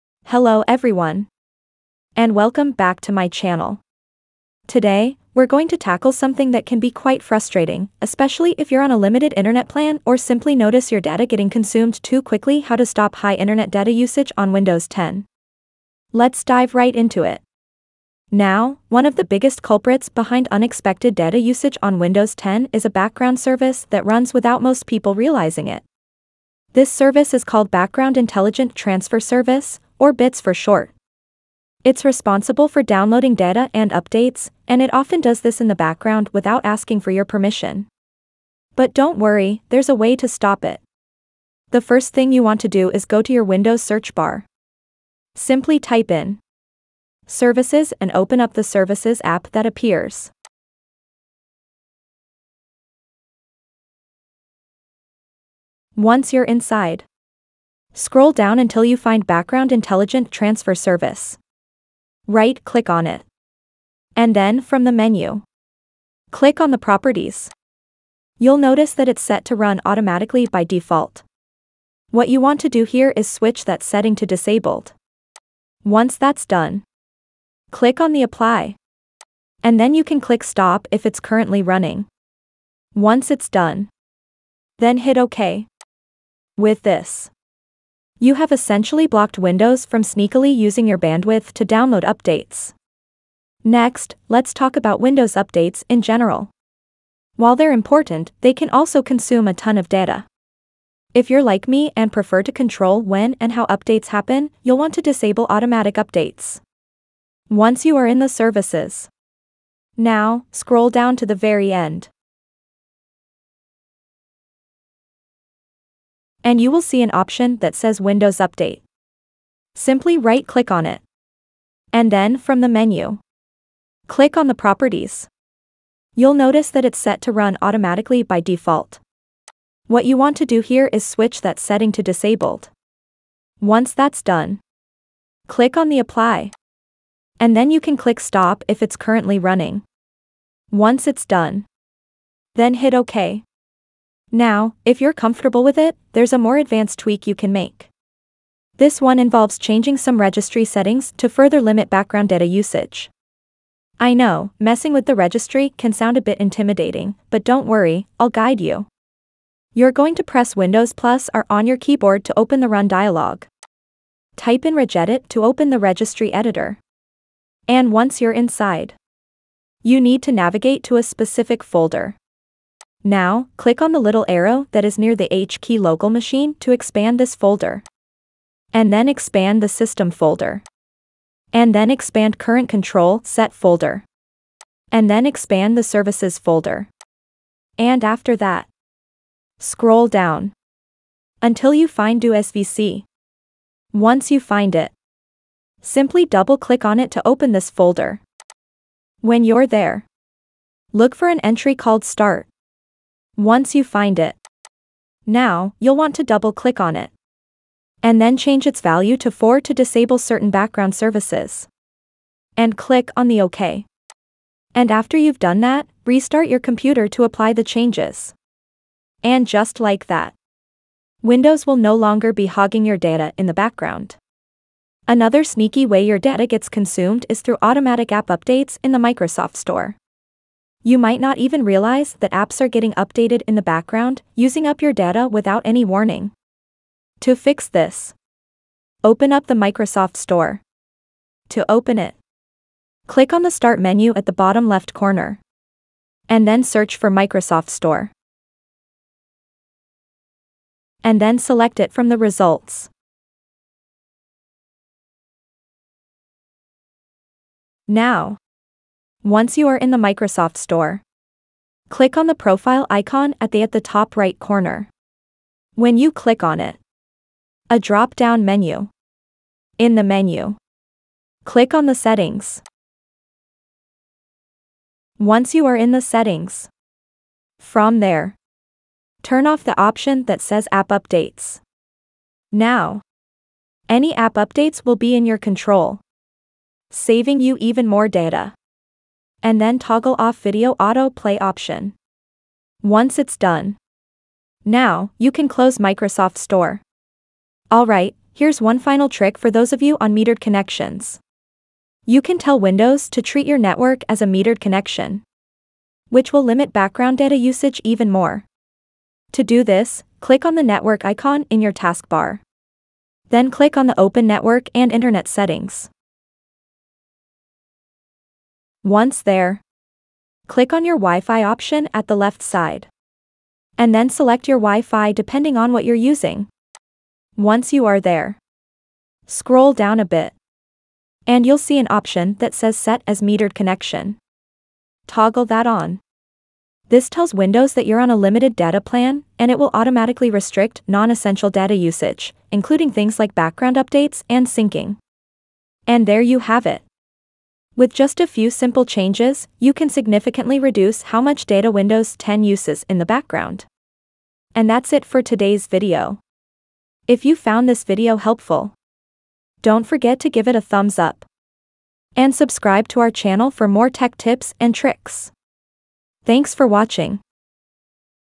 How To Stop High Internet Data Usage On Windows 10/11 | Stop Background Data Consumption (Laptop/PC)
Learn how to stop high internet data usage on Windows 10/11 by managing background data consumption on your laptop or PC. This tutorial covers practical tips to save internet data, reduce unnecessary data usage, and optimize your device's performance. Perfect for those looking to minimize bandwidth consumption!